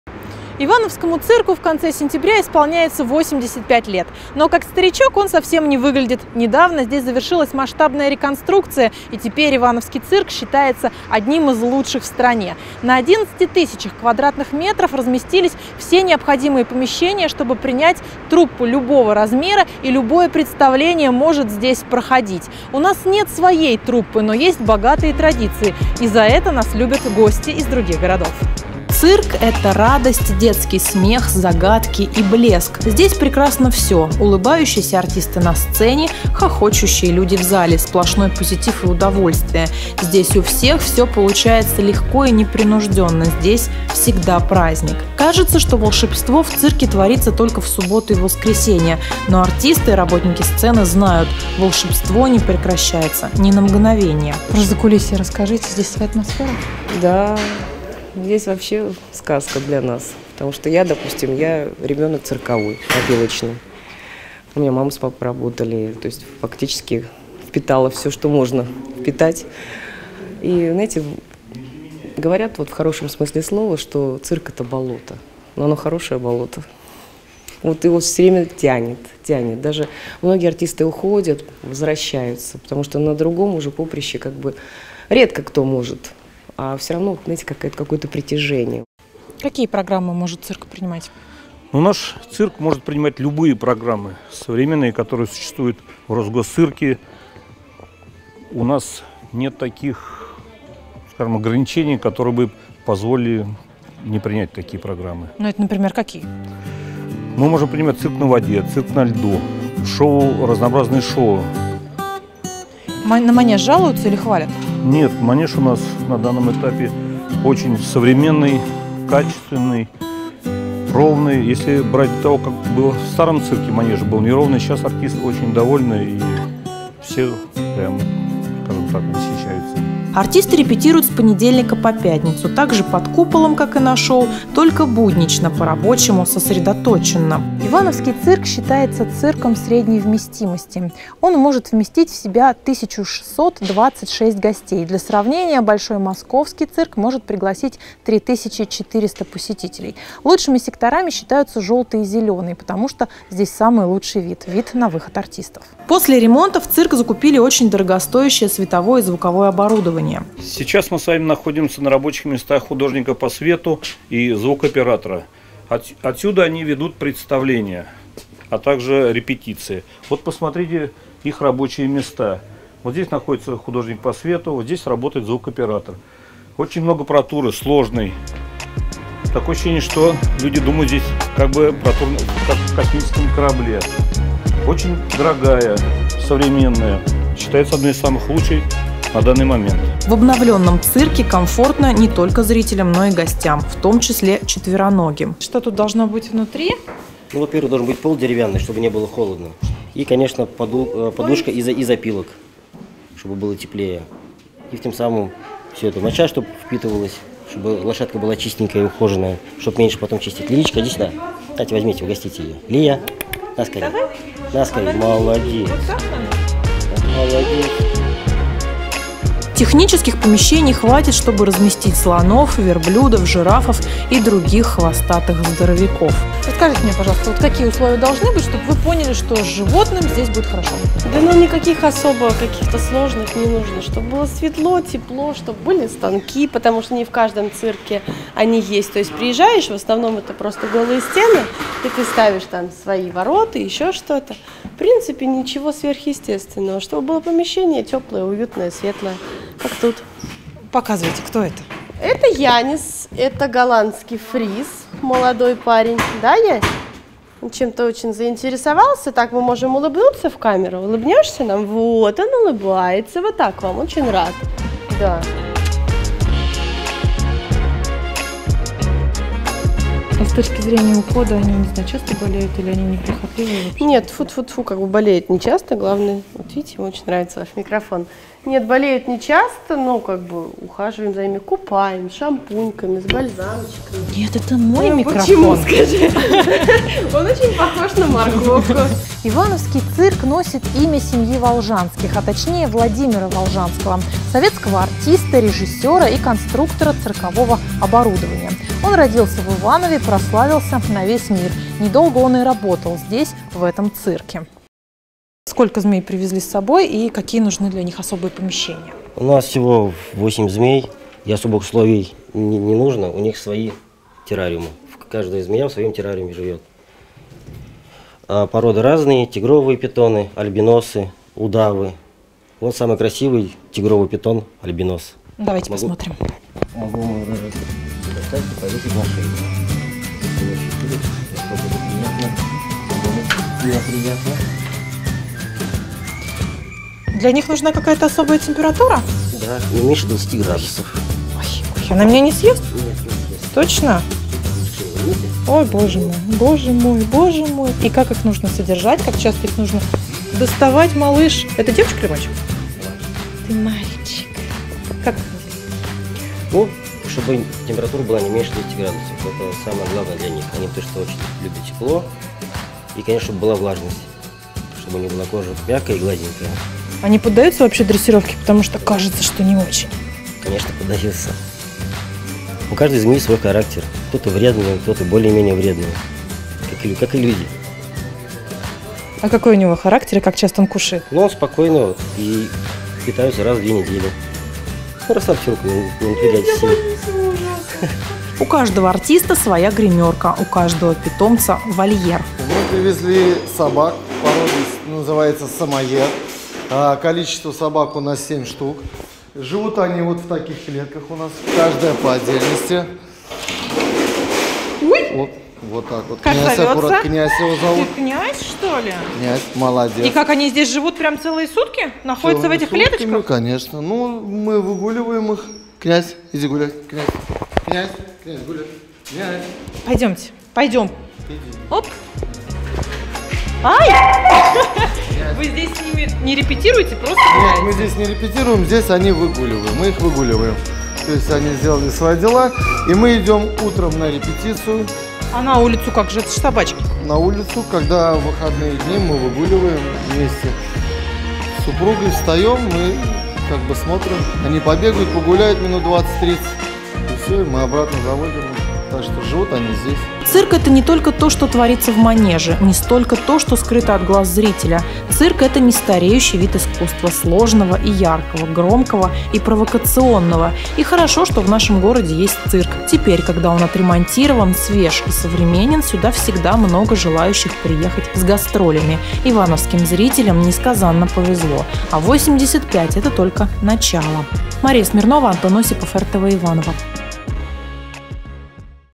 Цирковое закулисье
Цирк отмечает день рождения. Как выглядит закулисье и довольны ли гастролеры условиями – расскажем в поздравительном репортаже. При съемках материала ни один корреспондент не пострадал.